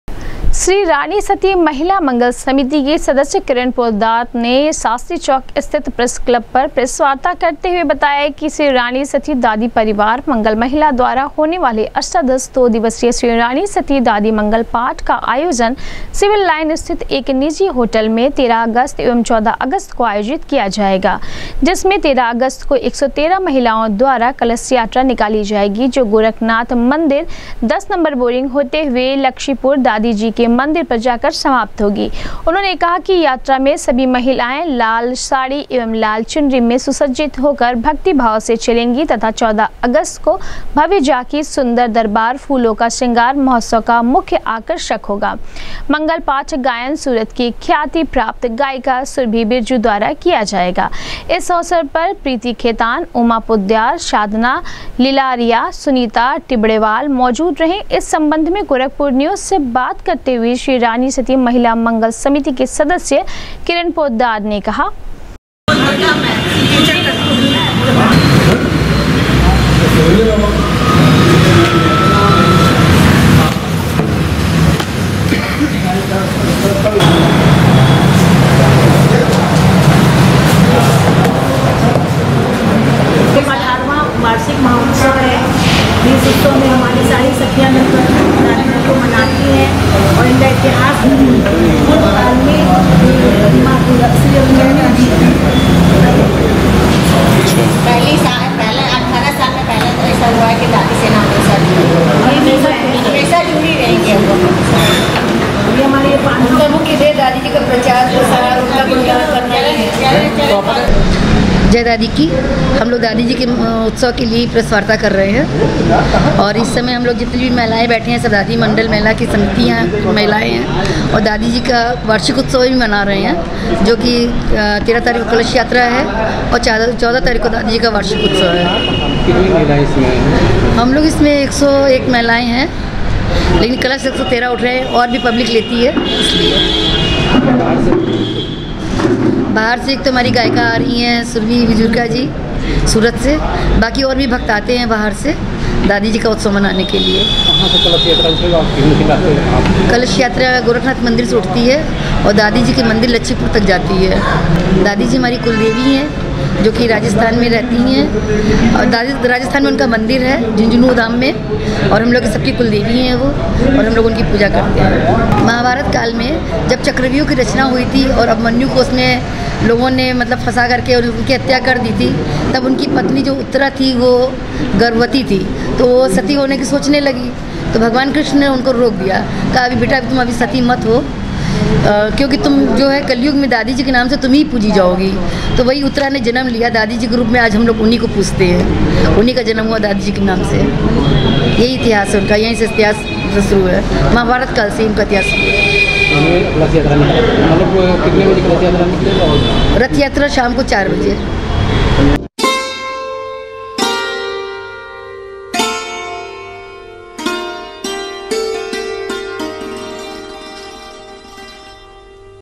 श्री राणी सती दादी परिवार मंगल महिला द्वारा अष्टादश दो दिवसीय श्री राणी सती दादी मंगल पाठ का किया
श्री राणी सती दादी परिवार मंगल महिला द्वारा अष्टादश दो दिवसीय श्री राणी सती दादी मंगल पाठ का किया गया आयोजन,कहा, 13 अगस्त को 113 महिलाओं द्वारा निकाली जाएगी भव्य कलश यात्रा, 14 अगस्त को सिंगार महोत्सव की निकलेगी भव्य झांकी